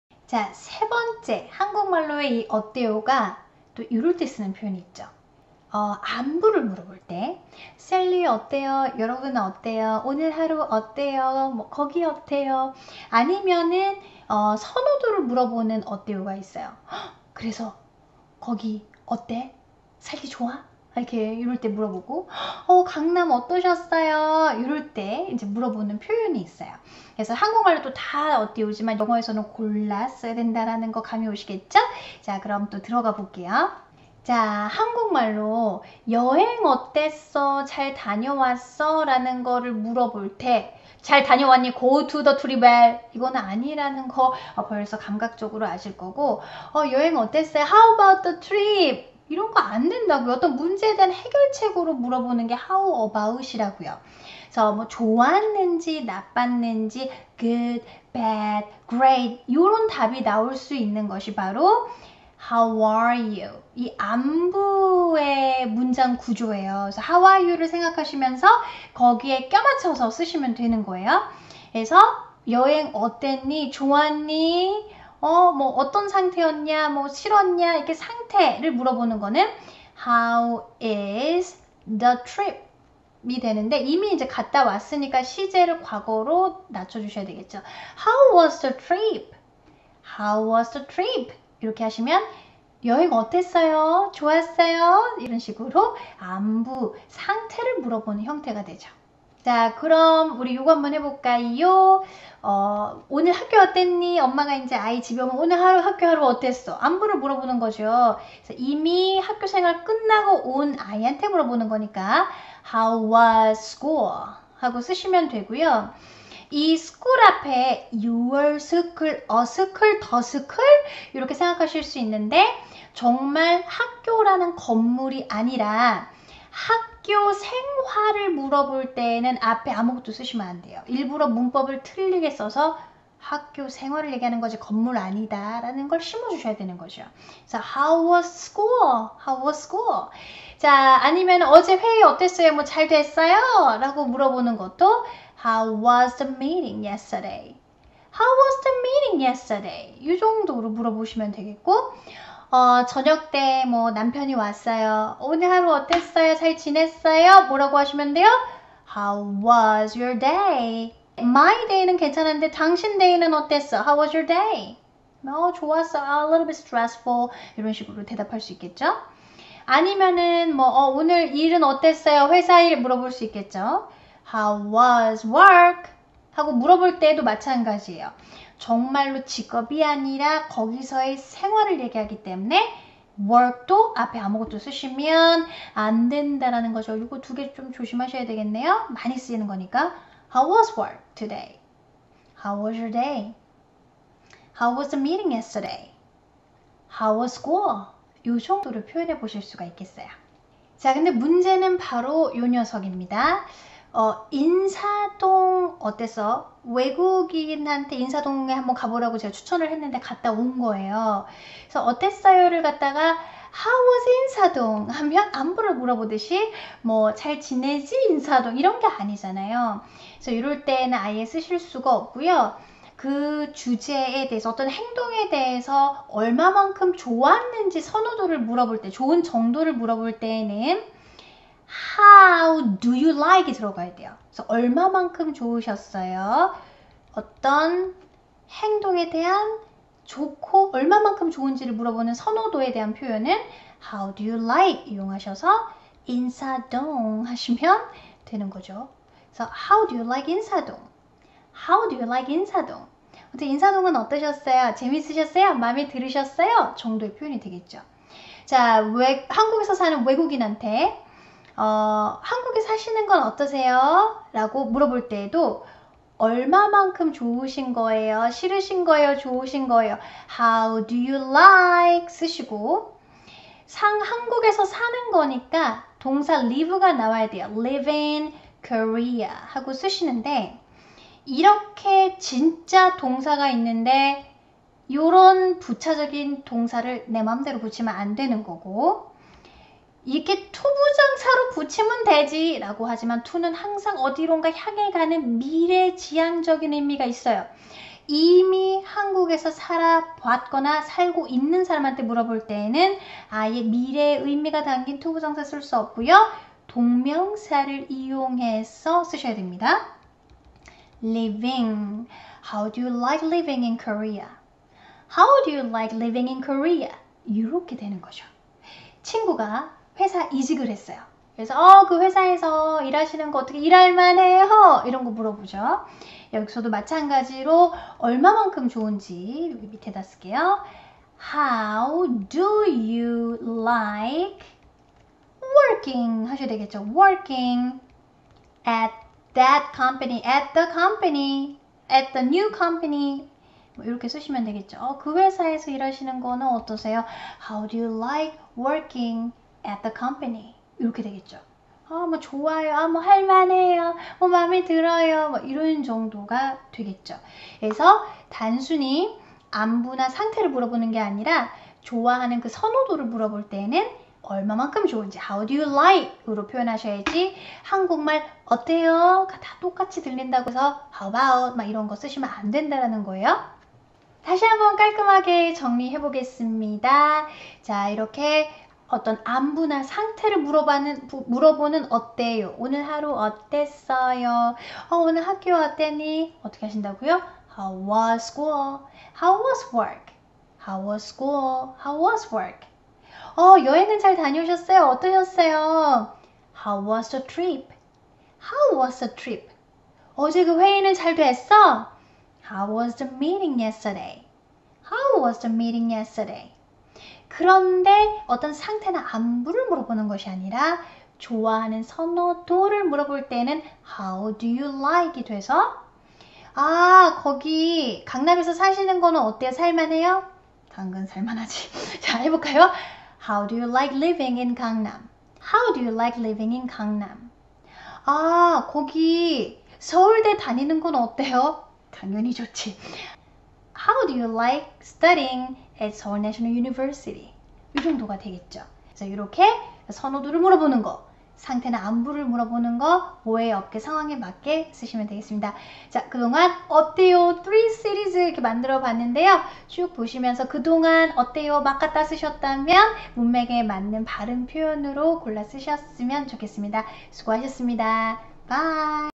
(ep.14) 한국인한테 너무 어려운 영어표현: how about/how is/How do you like의미차이ㅣ원어민영어ㅣ실전영어
여행 어땠어? 와  제주도는 어떠셨나요? 가 어떤 차이가 있는지 모르시는 분은 꼭꼭 시청해 주세요!
How is/was. ?
How do you like?의 표현 차이를 배웁니다.

12년 강남 스타강사로써 활약해온 Selley의
독보적인 컨텐츠로 확인해 보세요!

시청해 주시고 응원해 주시는 여러분들께
늘 감사하는 마음으로 영상제작하고 있습니다.
감사합니다!!